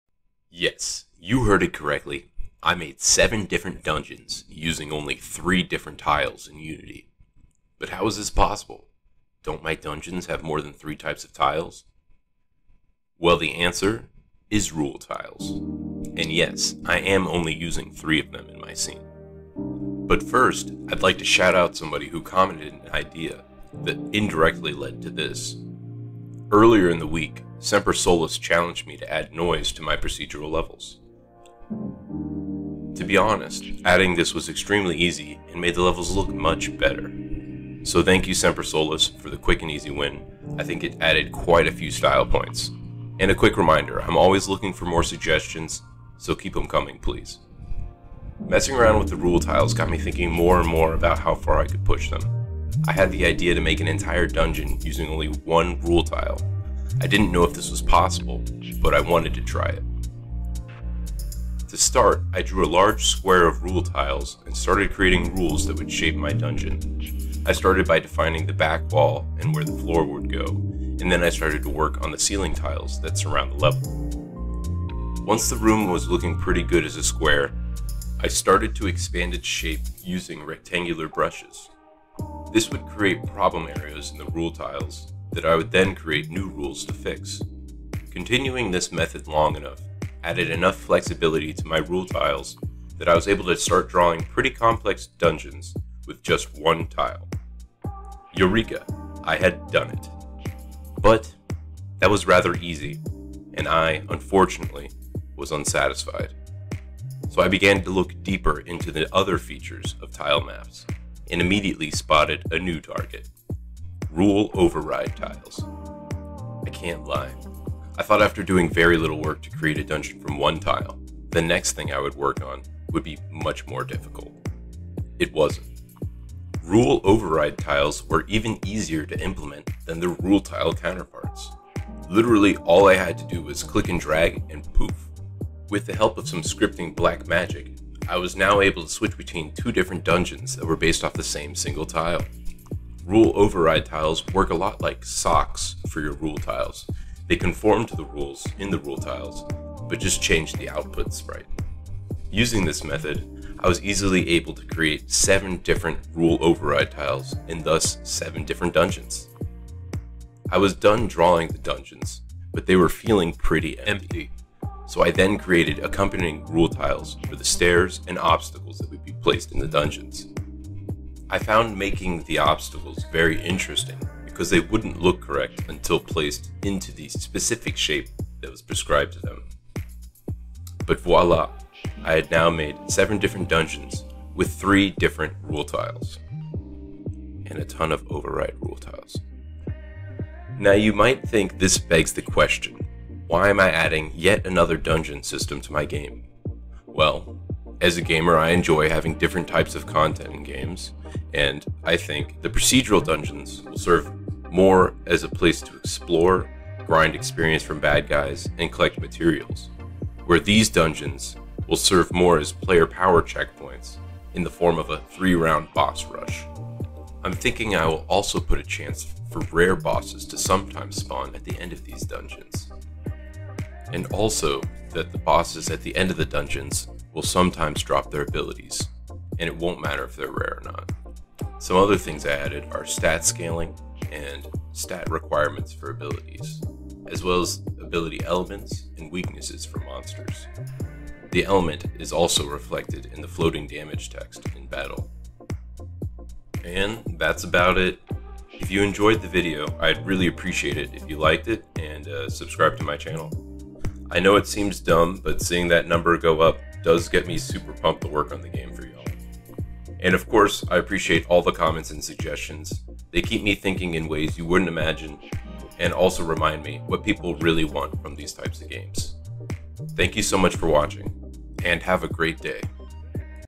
I Created An Entire Dungeon Only Using One Tile (And Seven Dungeons With 3 Tiles) - Unity Devlog #5
WISHLIST MY GAME ON STEAM! THANK YOU!

*One Rule Tile. Hey what's up! This video is another devlog for my game! In this video I talk about challenges, and how I challenged myself.
The Challenge:
To make the most with the least with Rule Tiles, and Rule Override Tiles in Unity!

Chapters:
00:00 Intro
00:20 Shoutout/Initial Challenge
00:49 Making a dungeon out of a single Rule Tile
1:46 A New Target: Rule Override Tiles
3:10 More Dungeons?
3:51 Other Things I added
4:08 Outro